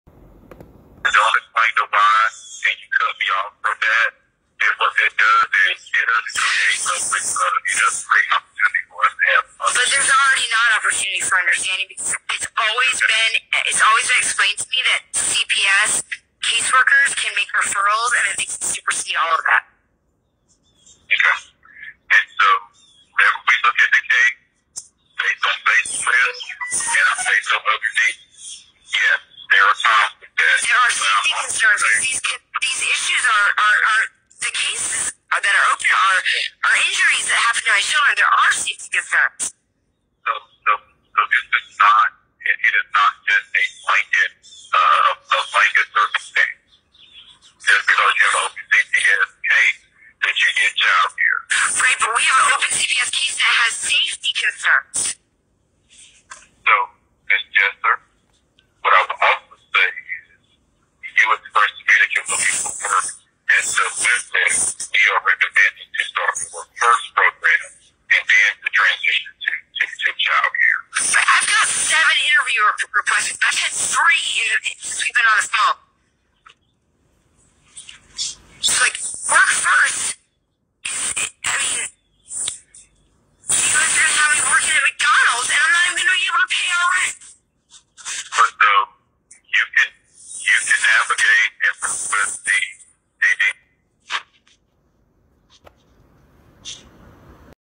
Victor Isler, Forsyth Department of Social Services Director, Argues
he denies there are safety concerns and attempts to trap the parent into a program for entry level jobs. The program "Start to Work First" is a North Carolina program funded under TANF, a federal program which provides money to states.

In this interview, Isler denied that federal incentives incentivized taking kids or placing people into programs,

Also, Isler claims that DSS cannot merely override a court order, but in this audio, his caseworker, Kent Webb, claims DSS can switch custody from one parent to another by fiat.